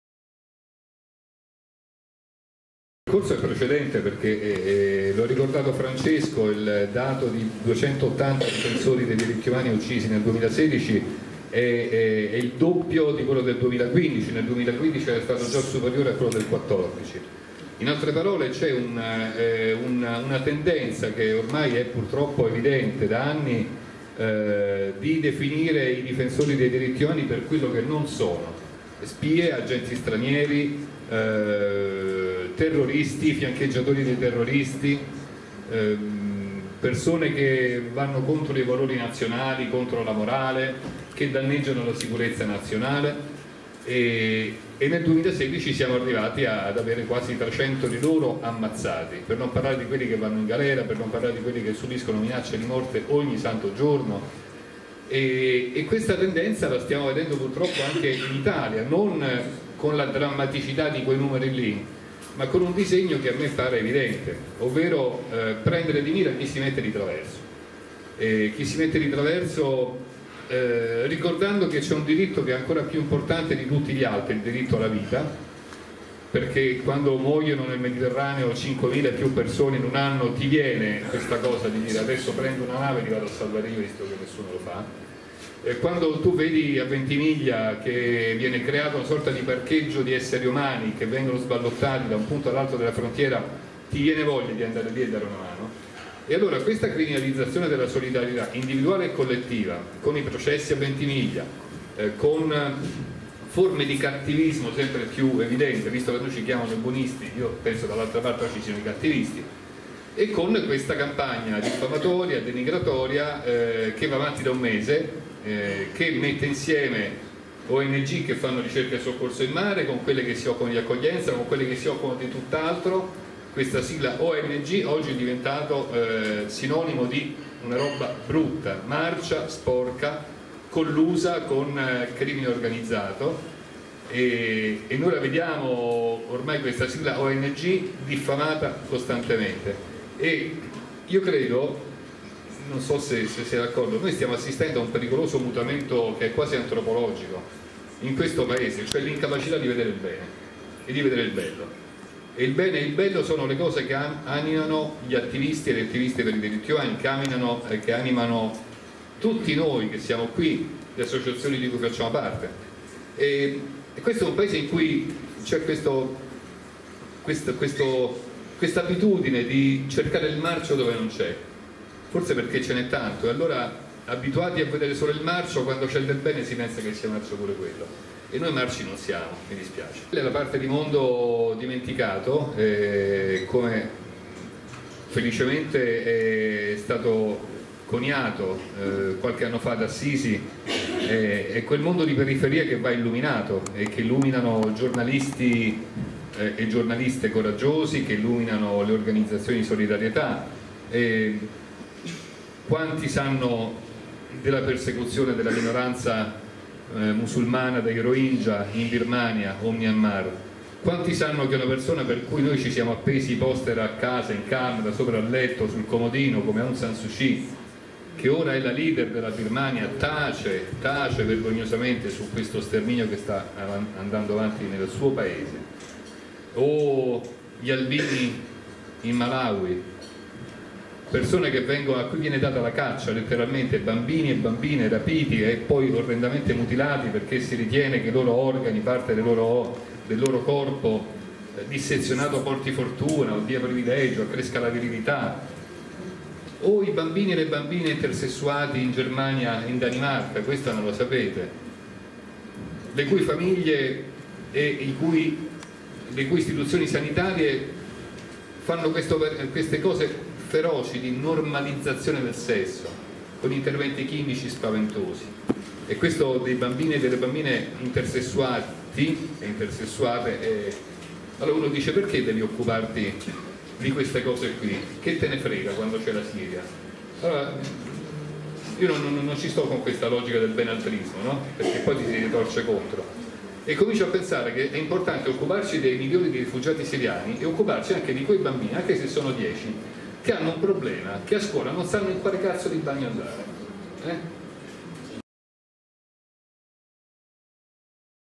19.05.17 Ai confini dell'umanità - Riccardo Noury Amnesty International (2)
Intervento realizzato durante la serata sui diritti umani